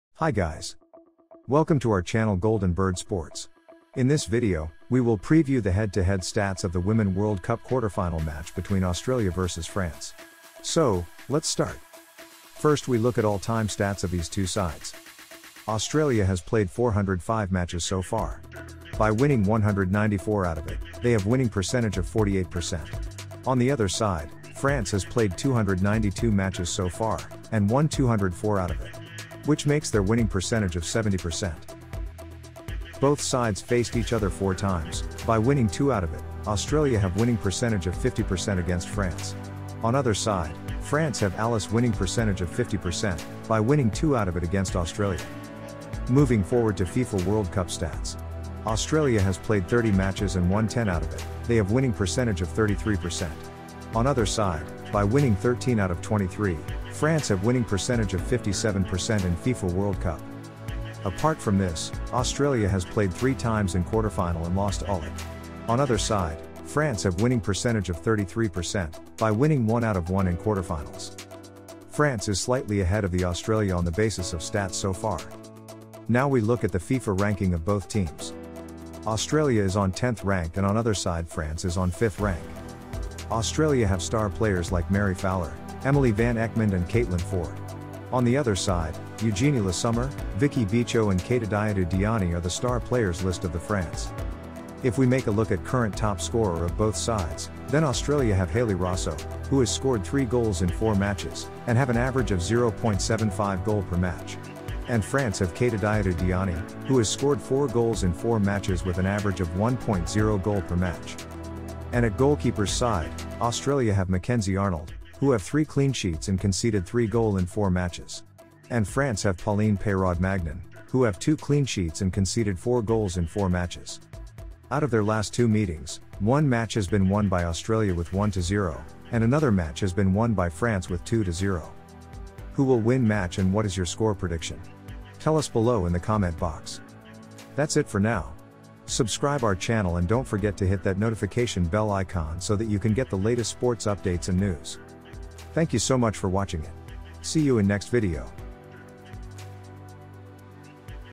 AUSTRALIA vs FRANCE | QUARTERFINALS | Head to Head Stats | FIFA WOMEN'S WORLD CUP 2023
AUSTRALIA vs FRANCE Head to Head
AUSTRALIA vs FRANCE Live Score
AUSTRALIA vs FRANCE Match Preview
AUSTRALIA vs FRANCE WORLD CUP